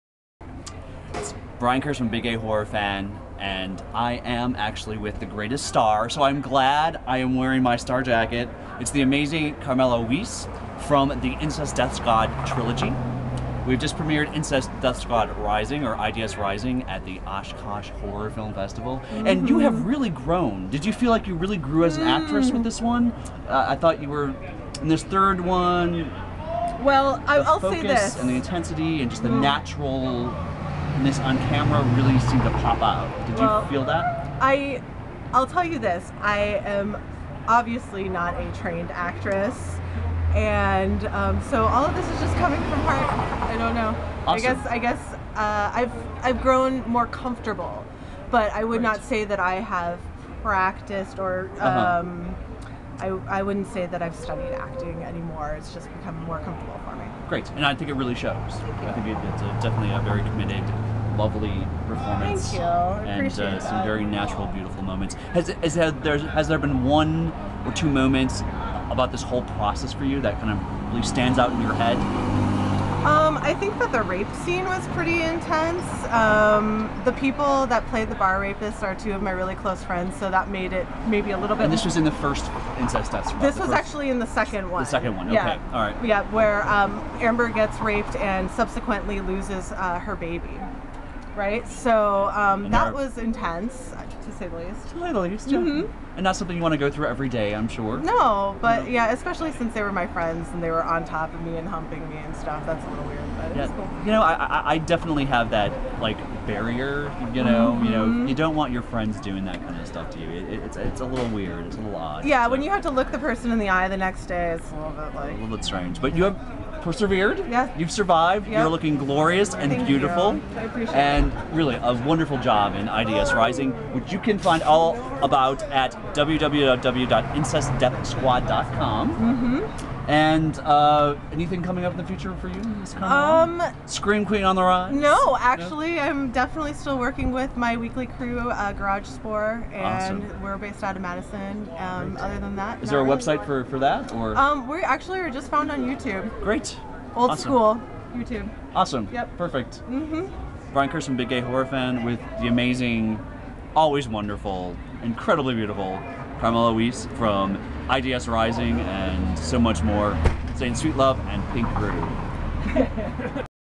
Big Gay Horror Fan Interviews IDS Rising's Carmela Wiese!
BGHF talks with the amazing Carmela Wiese, the scintillating star of the Incest Death Squad trilogy at the Oshkosh Horror Film Festival 2012.

Captured on camera by the ever amazing Chad Hawks, of Studio Boo.